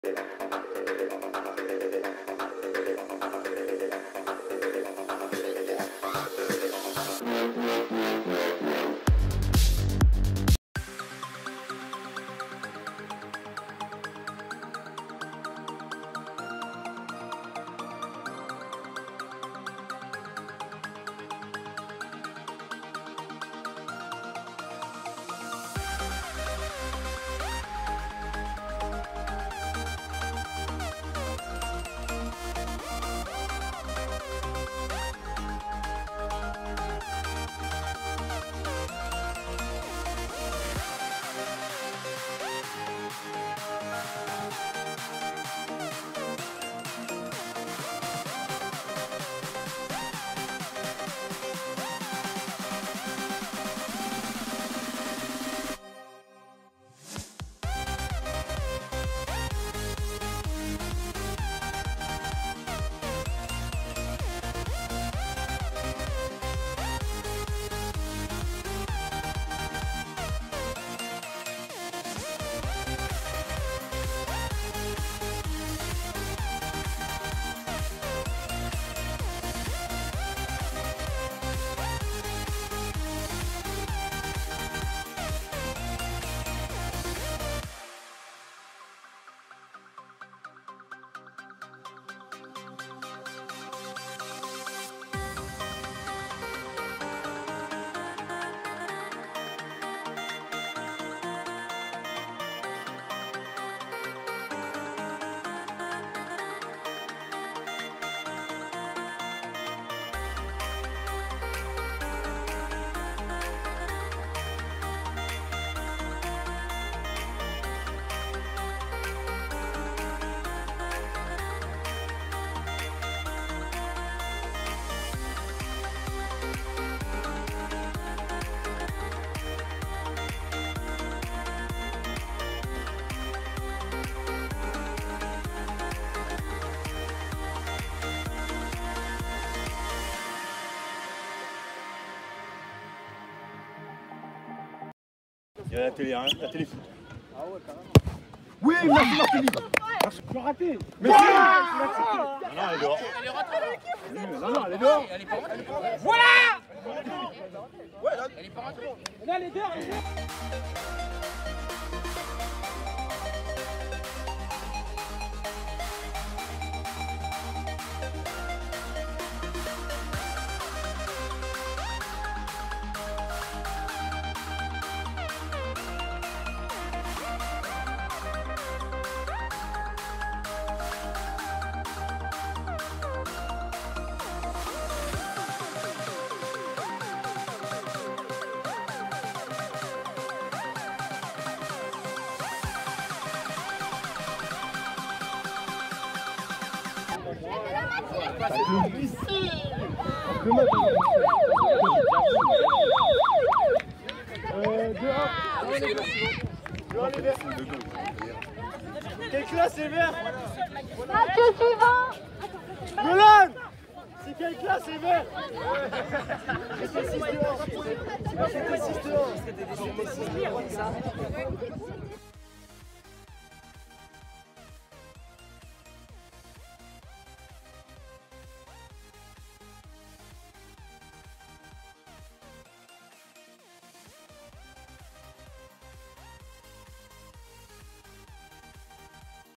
Best Of Olympiades 2022
Petit montage à partir des rushes récoltés dans le cadre du tournage du journal WebTV sur les Olympiades 2022

Un montage signé Enzo Roudie-Vassal